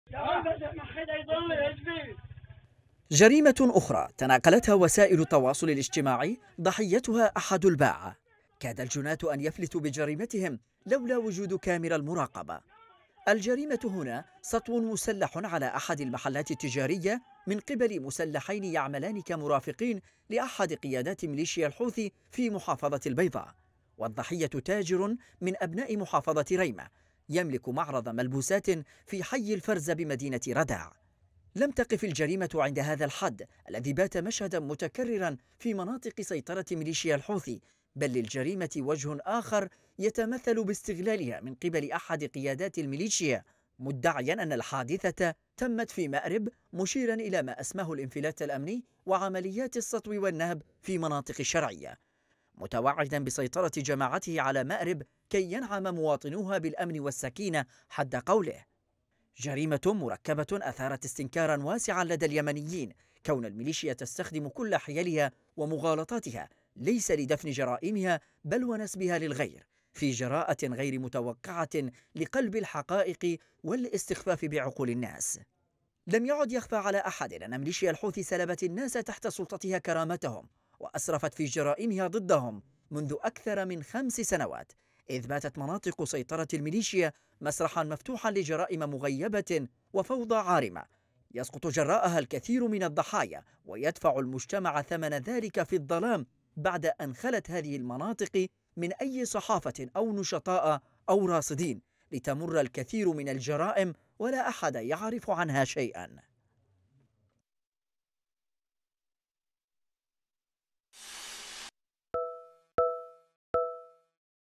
الجريمة المغيبة في سلطة المليشيا | تقرير: منصور النقاش
مكان وقوع الجريمة مدينة رداع..  المجرم اثنان من مرافقي أحد قيادة المليشيا..الضحية: تاجر من ريمة.. كيف يمكن لمليشيا الاستخفاف بالناس وترويج الأكاذيب والمغالطات ونشرفيديو الجريمة باعتبارها حصلت في مأرب